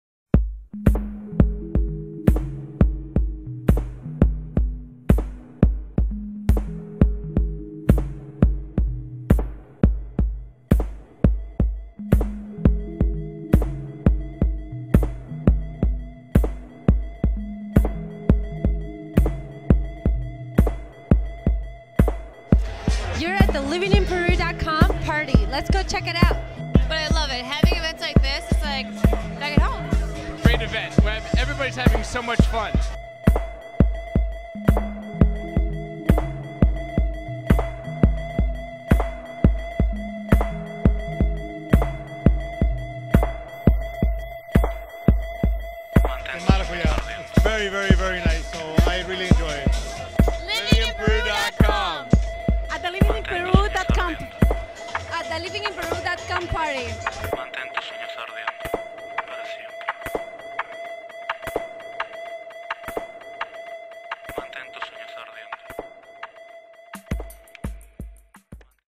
Lima Party with LivinginPeru.com
Chilcano Night at Mayta was the latest social gathering organized by LivinginPeru.com and Excelan Education in Lima, Peru.